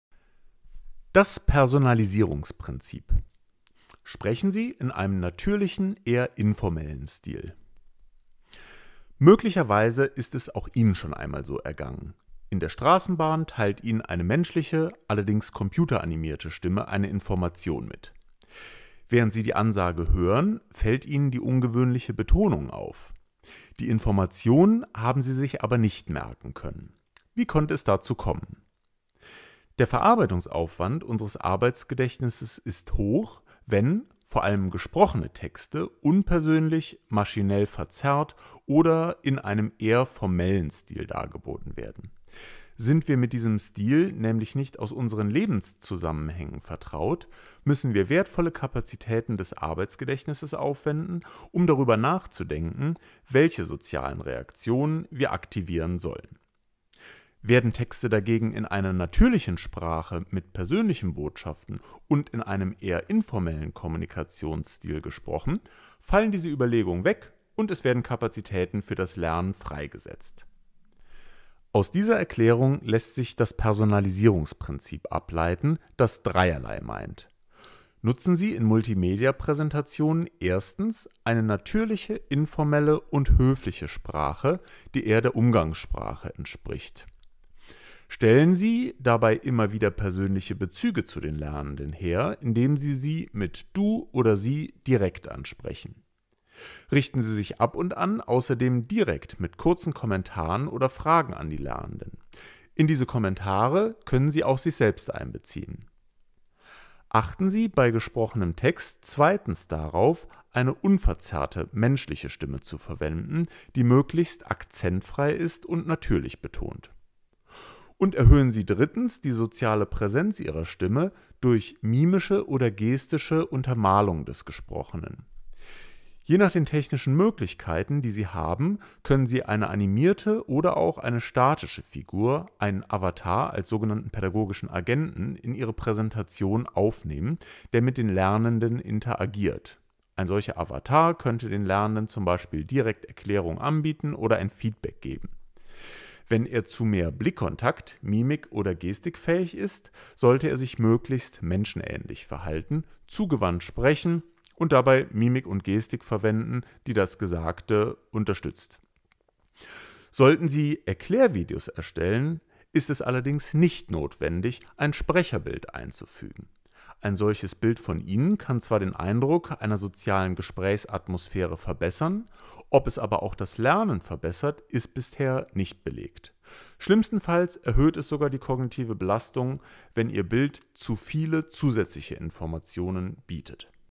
Personalisierungsprinzip
In diesem Video wird das Personalisierungsprinzip auf Basis der kognitive-load Theorie erklärt. Das Video ist Bestandteil des Online-Selbstlernkurses "Erstellung lernförderlicher Arbeitsmaterialien nach der kognitive-Load Theorie". Weitere Informationen finden sie im Fachbereich 35 des NLQ Hildesheim

Das Video wird unter der Lizenz CC-BY 4.0 veröffentlicht. Für die Namensnennung soll genannt werden: Niedersächsisches Landesinstitut für schulische Qualitätsentwicklung (NLQ)